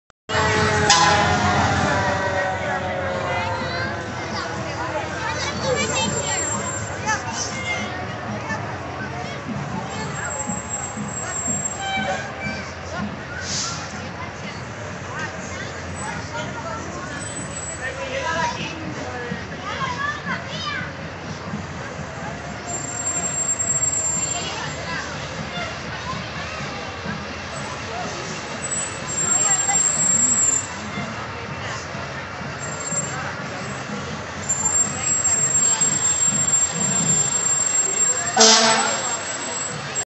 Neversink Fire Company Units in Lebanon County Holiday Parade
Neversink Fire Company (@NeversinkFireCo) engines 14 and 14-1 make their way down the parade route during the 47th annual Lebanon County Holiday Parade in downtown Lebanon, PA. Neversink Fire Company provides fire protection in North Cornwall Township, PA.